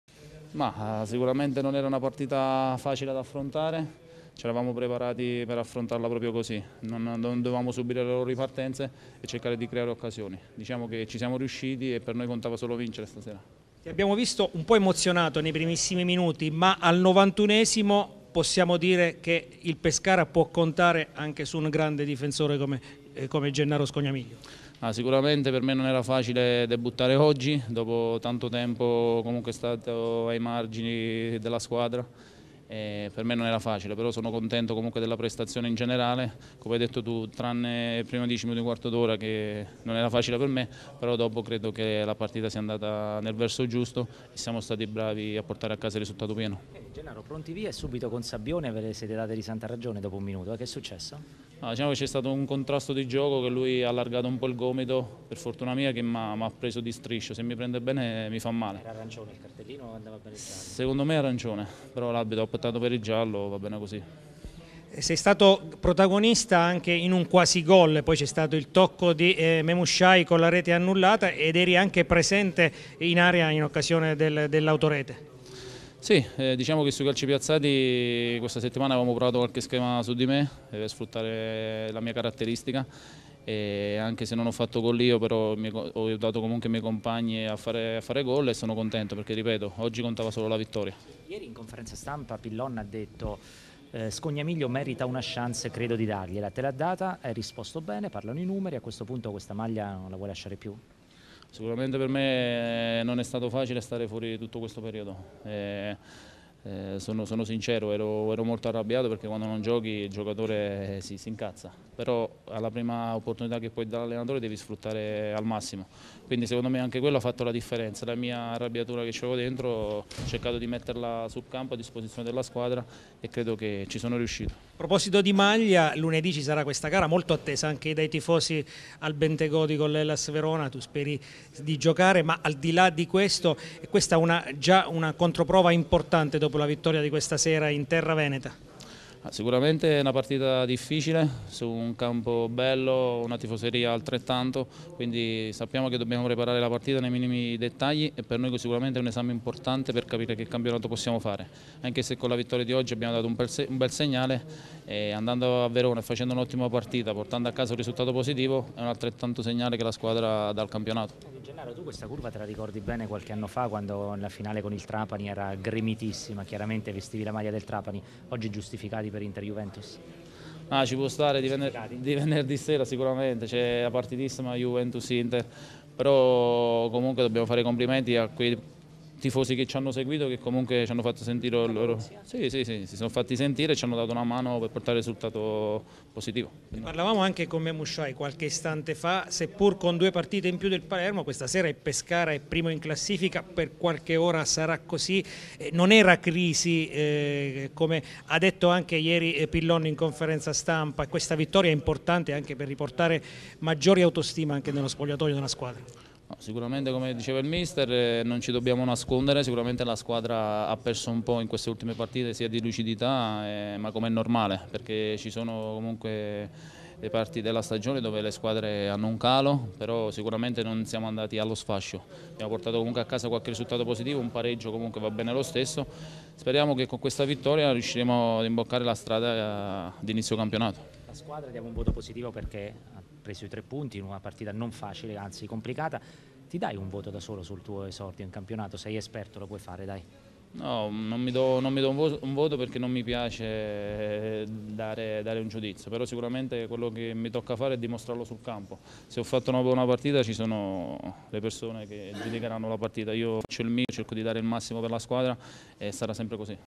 Pescara - Carpi 2-0: Gennaro Scognamiglio
Intervista post gara al difensore del Pescara, al debutto in campionato